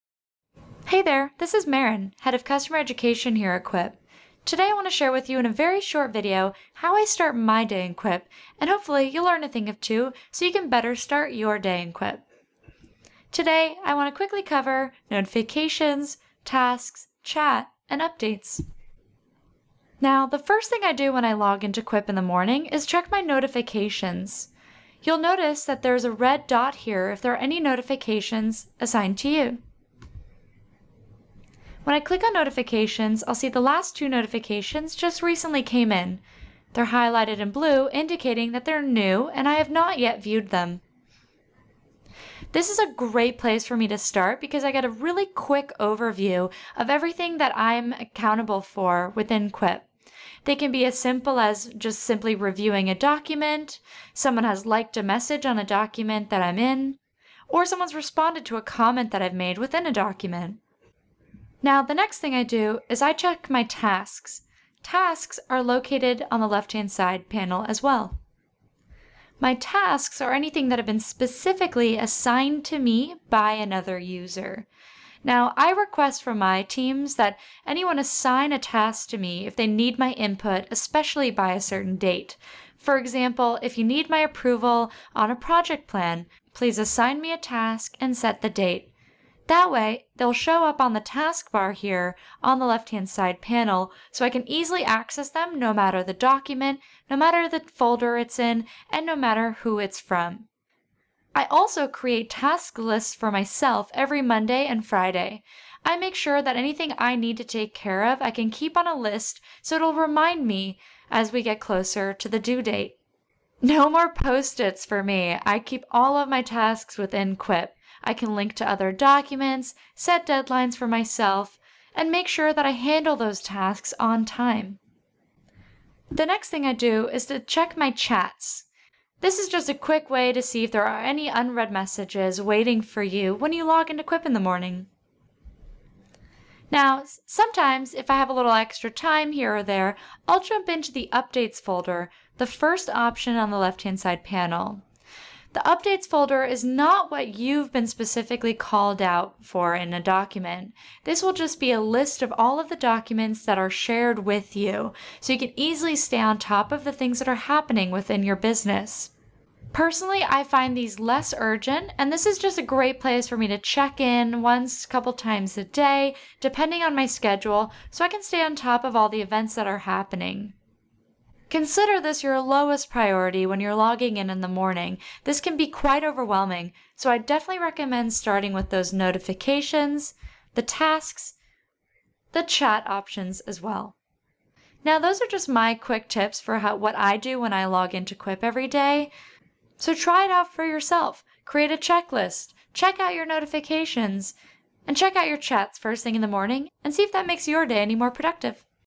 Quip EDU: How I start my day in Quip
In this video, we'll look at how you can start your day more effectively in Quip by accessing notifications, tasks, chat and updates!